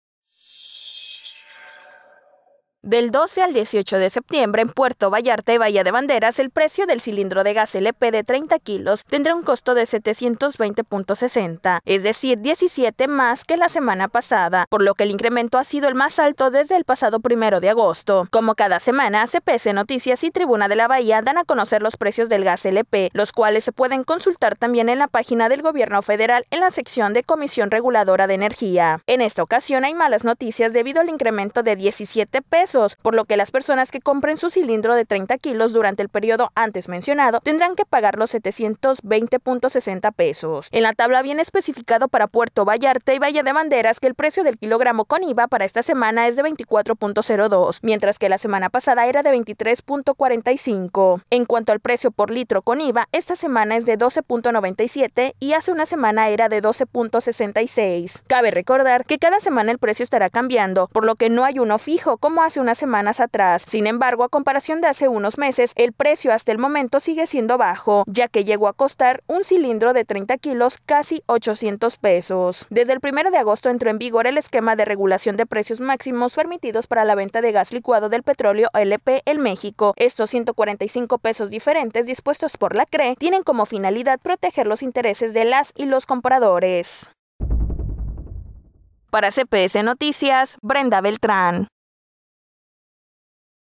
Sube la 4T el gaaaas 17 pesos | CPS Noticias Puerto Vallarta
Del 12 al 18 de septiembre, en Puerto Vallarta y Bahía de Banderas, el precio del cilindro de gas LP de 30 kilos tendrá un costo de $720.60.